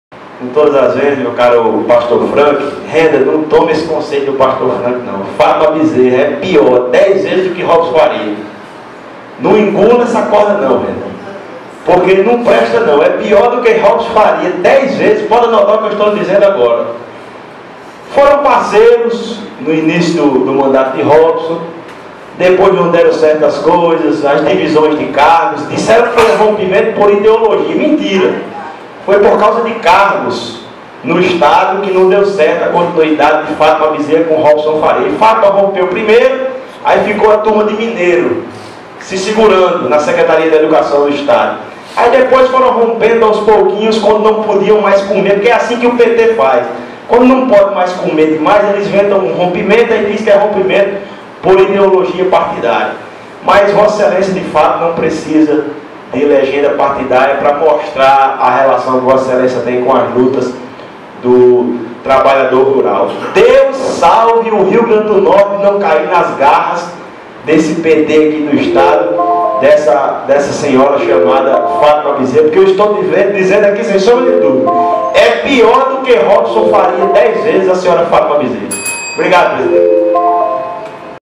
Vereador Fernando Guilherme(DEM), Fátima Bezerra é 10 vezes pior que Robson Fárias.
Blog do Jasão  João Câmara/RN